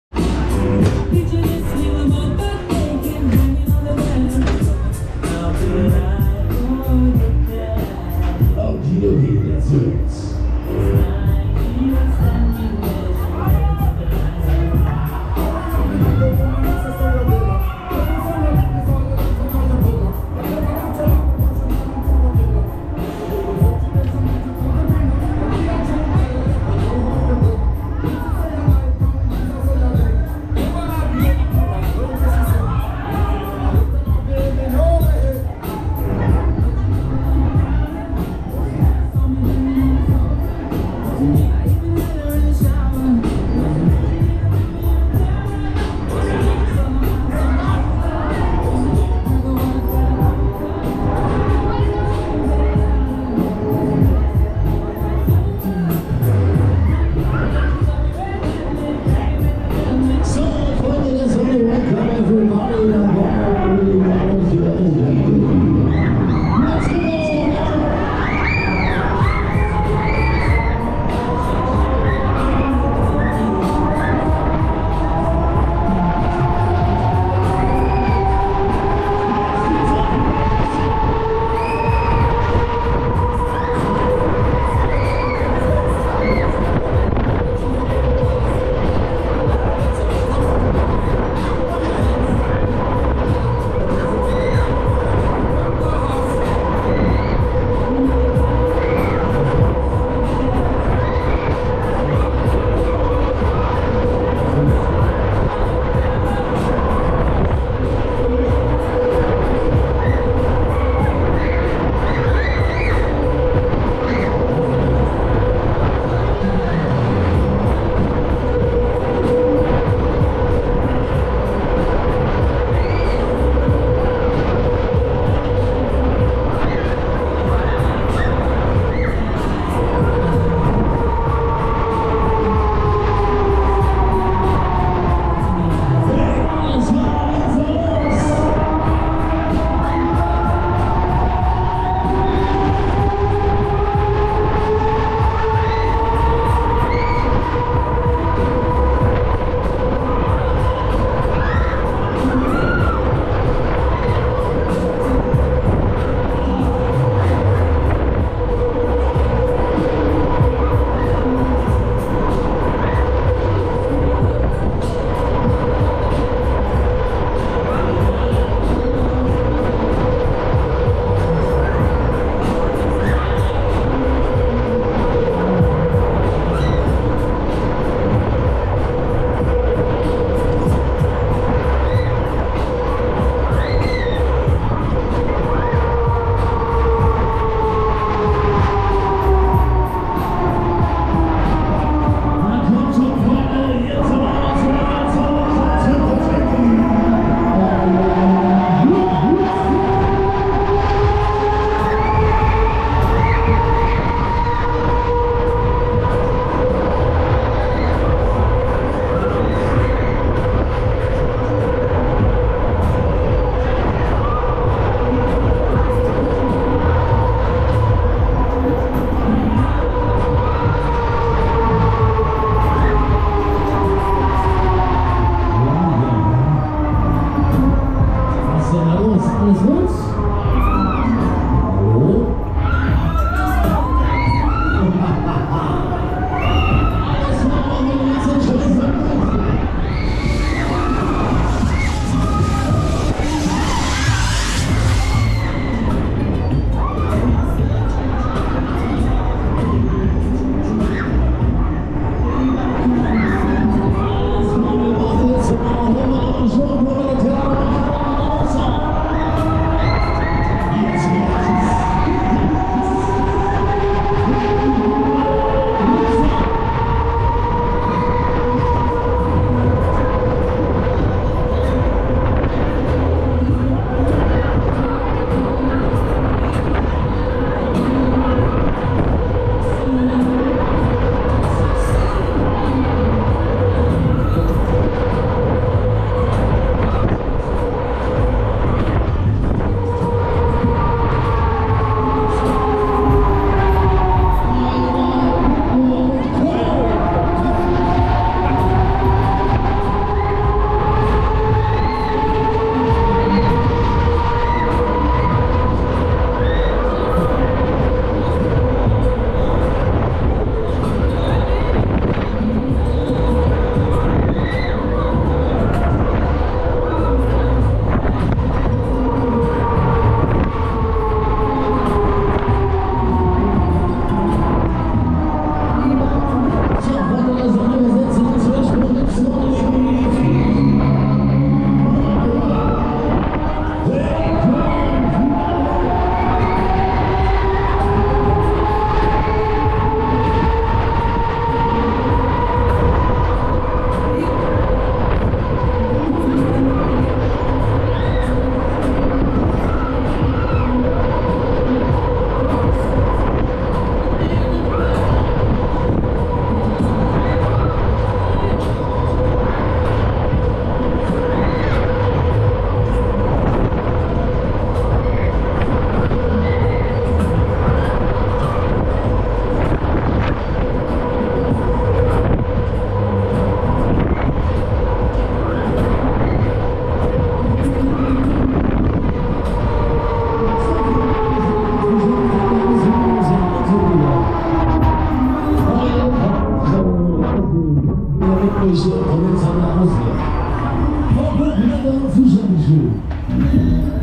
⁴ᴷ Break Dance No2 - Bonner - Onride | Palmkirmes Recklinghausen 2023
Break Dance von Bonner Onride Video von der Palmkirmes in Recklinghausen 2023.
Huss Break Dance 2 Breakdancer Breaki Kirmes Volksfest Rummel Palmkirmes Recklinghausen 2023.

Schausteller: Bonner Schaustellerunternehmen e.K. (Witten)
Hersteller: Huss (Bremen)
Typ: Break Dance 1 (Nr. 3)
Baujahr: 1993
Fahrpreis: 4,00€